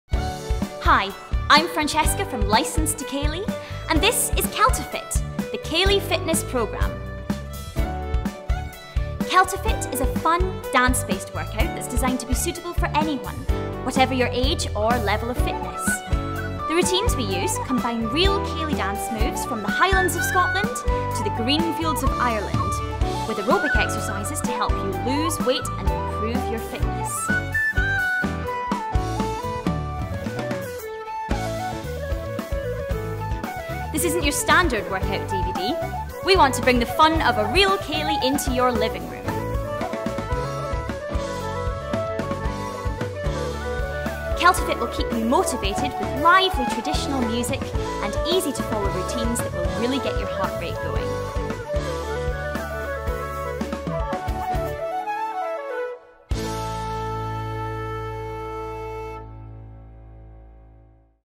Kelta Fit: Fitness From Scotland - Official Promo Video
Kelta Fit is the new fitness DVD / Download that brings you fitness from Scotland!

It's a fun and unique dance workout that incorporates Scottish ceilidh dance moves and uses an original celtic soundtrack!

It's a dance based workout that's designed to be suitable for anyone, whatever your age or level of fitness. We combine real ceilidh dance moves from the highlands of Scotland and the green fields of Ireland, with aerobic exercised to help you loose weight, tone up and improve your overall health.

This isn't your standard workout DVD! Kelta Fit brings the fun of a real ceilidh into your living room and helps you get in shape, all at the same time!

We'll keep you motivated with lively celtic music you can't help but dance to, and the easy to follow moves will really get your heart rate going!